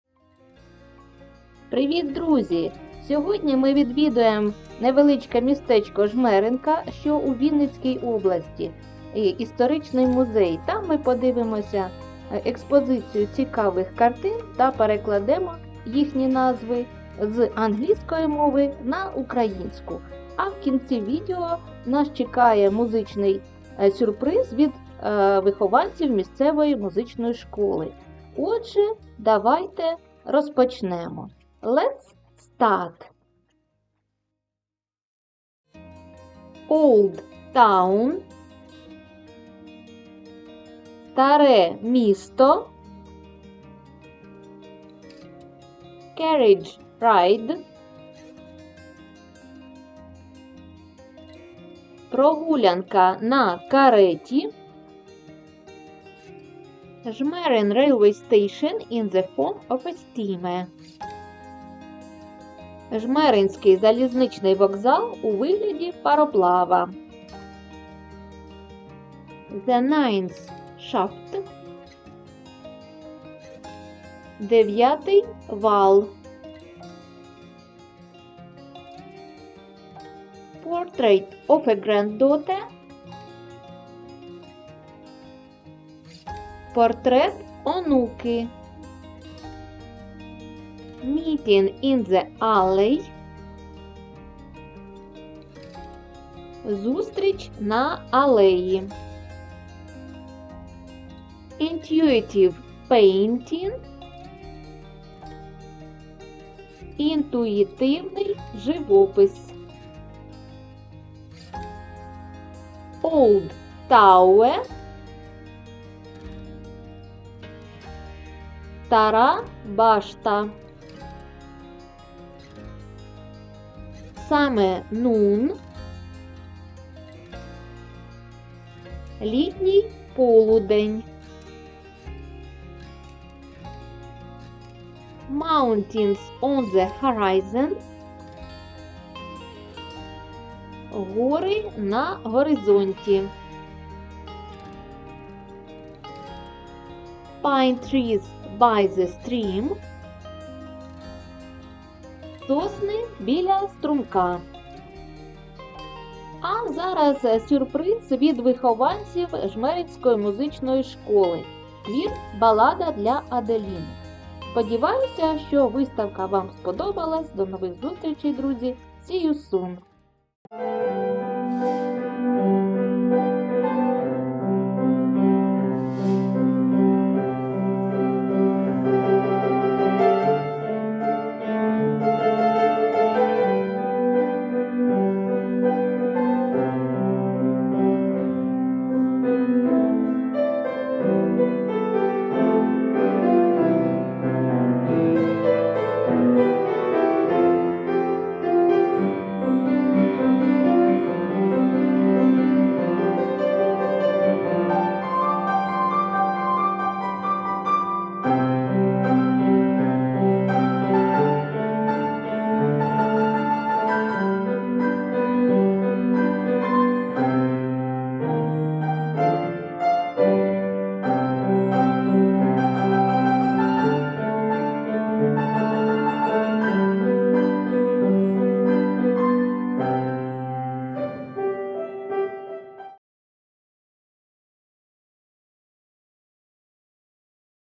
Картини у м. Жмеринка (Вінницька обл.)+ музичний сюрприз
Сьогодні я запрошую Вас у містечко Жмеринка у Вінницькій області. Ми побачимо цікаві картини та перекладемо їхні назви з англійської на українську. А в кінці відео нас чекає музичний сюрприз!